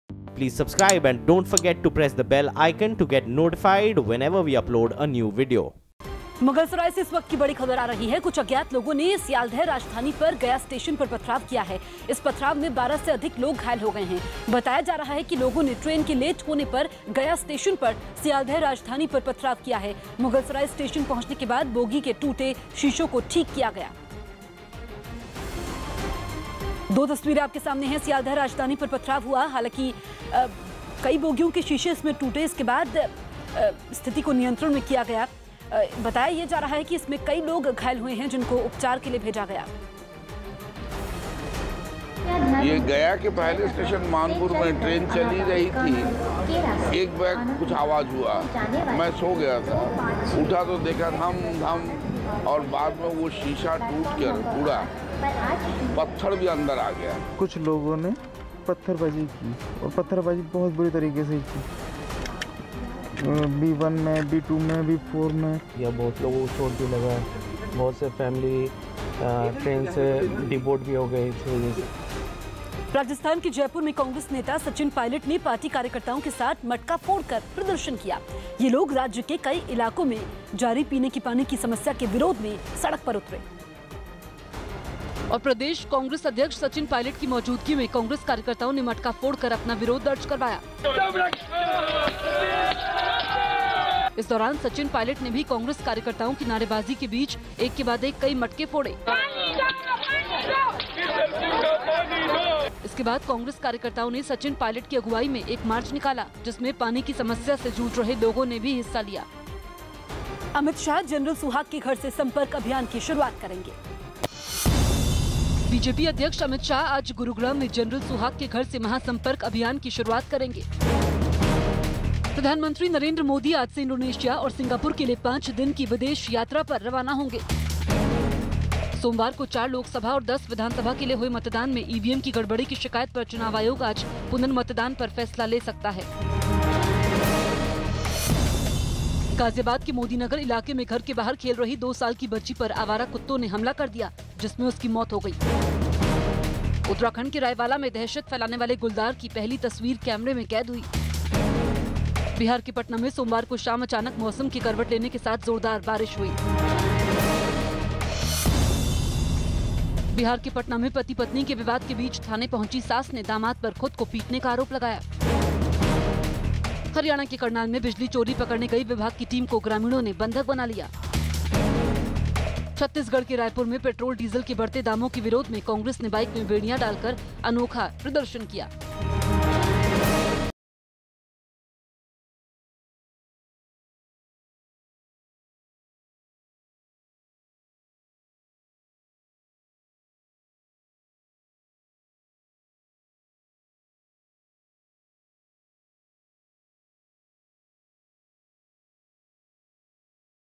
Sealdah Rajdhani Express के कई यात्री घायल | Gaya में राजधानी ट्रेन पर पथराव | News18 India
बिहार के गया में मानपुर स्टेशन के पास सियालदाह राजधानी एक्सप्रेस पर कुछ बदमाशों द्वारा पत्थरबाजी करने की खबर है. इस वारदात में सियालदाह राजधानी एक्सप्रेस के कम से कम 6 यात्री घायल हो गए हैं. अज्ञात बदमाशों द्वारा की गई पत्थरबाजी से राजधानी एक्सप्रेस की कई बोगियों के शीशे भी टूट गए. हालांकि अभी इस घटना के बारे में विस्तृत जानकारी उपलब्ध नहीं है.